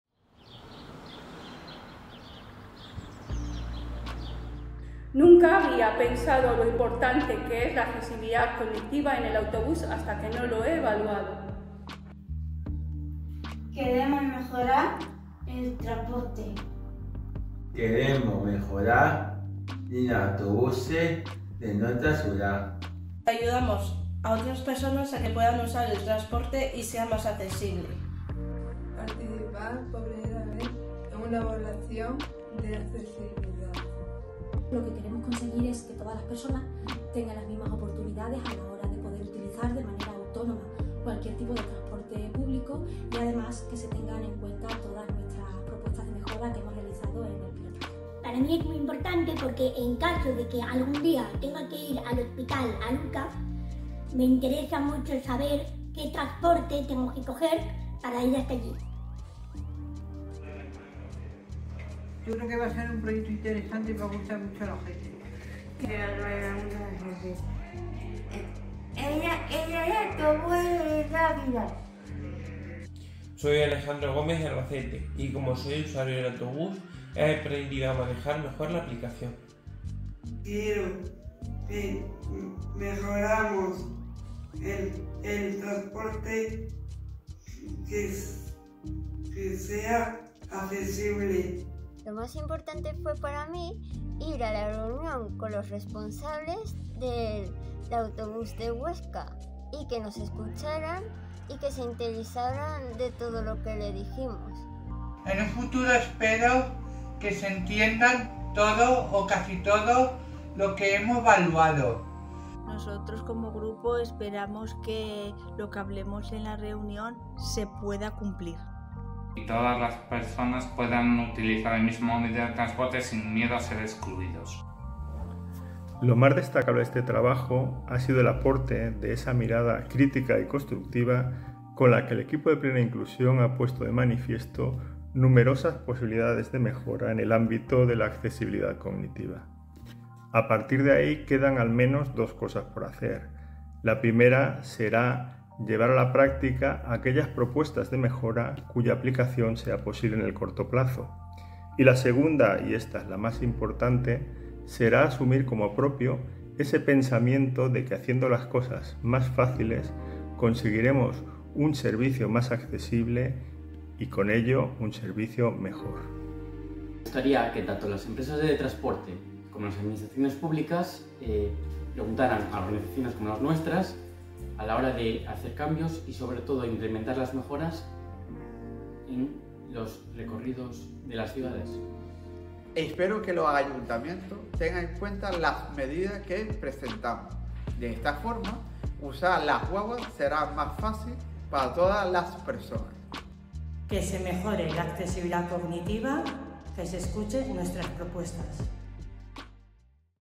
Comunidades accesibles | Plena inclusión
En un encuentro online
se ha presentado el proyecto
‘Comunidades Accesibles’,
una llave para la vida en comunidad,
en el que participaron 14 federaciones
de Plena inclusión.

El objetivo del proyecto
es promover procesos de transformación
e innovación social a través
de la accesibilidad cognitiva
para lograr comunidades
más accesibles e inclusivas.